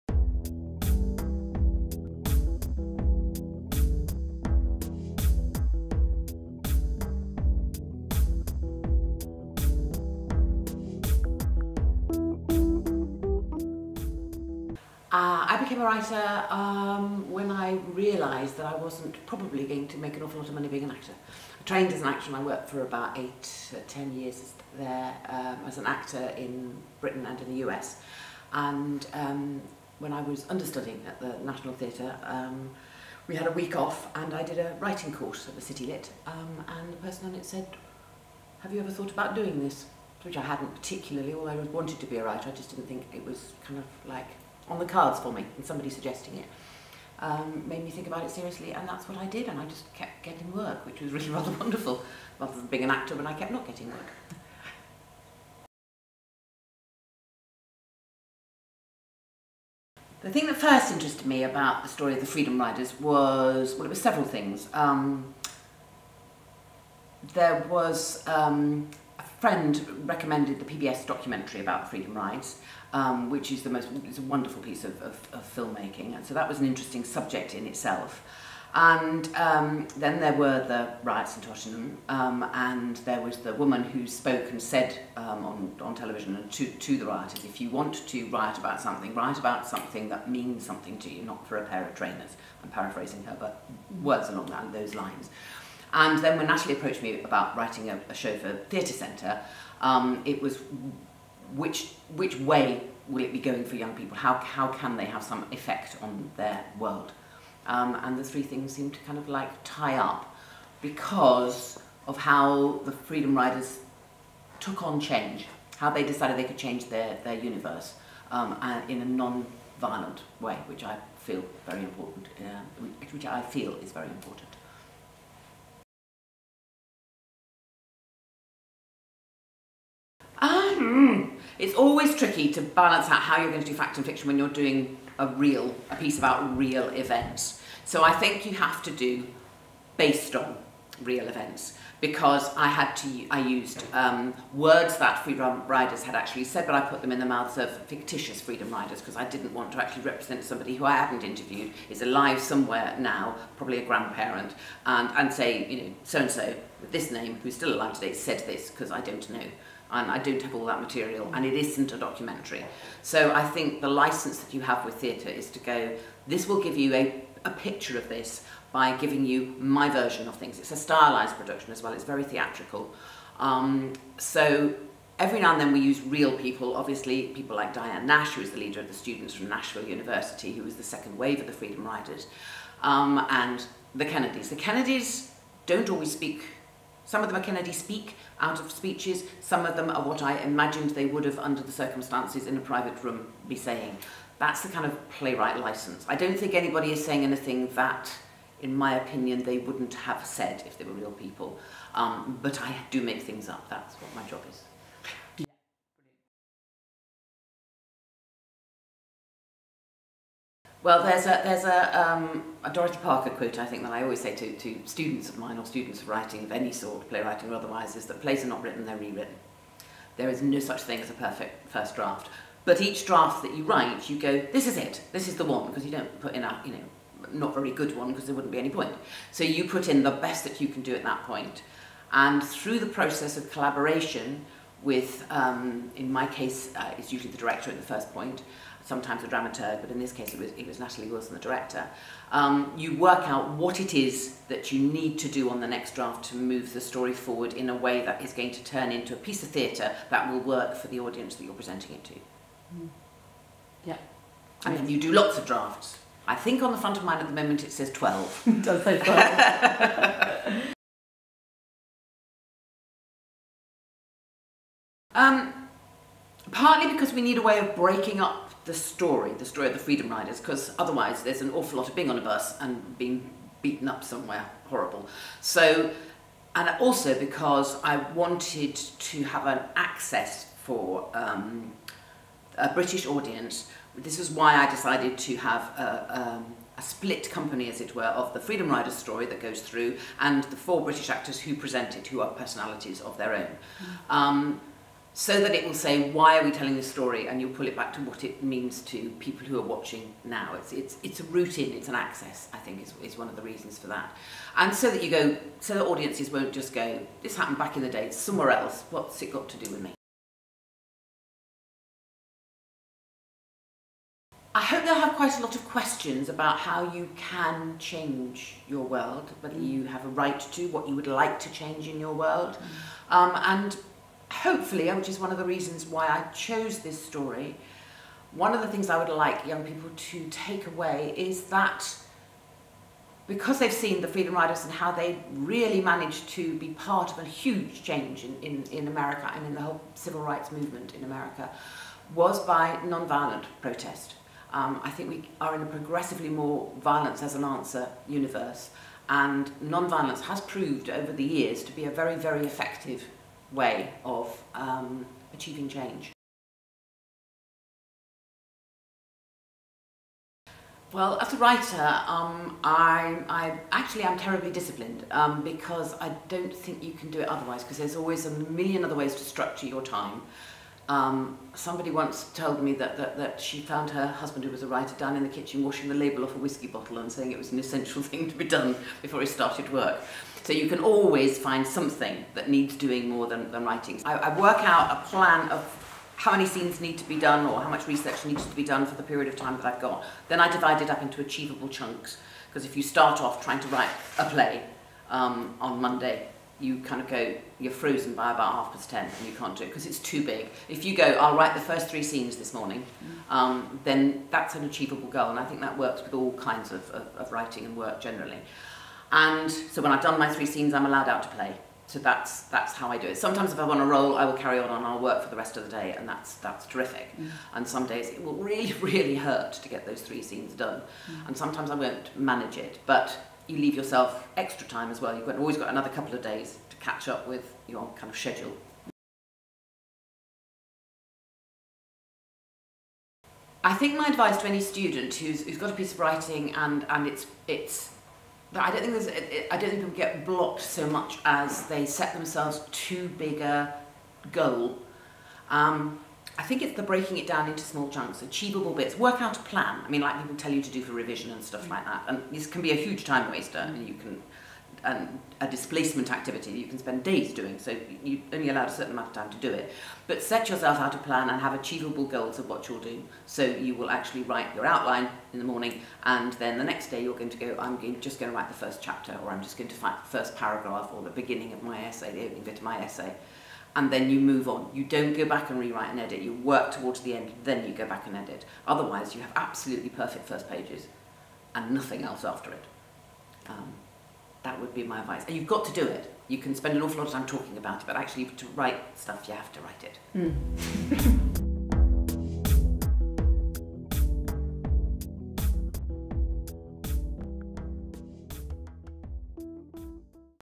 Rise Up: playwright Lisa Evans Q&A
Rise Up playwright Lisa Evans answers questions about the play and her career.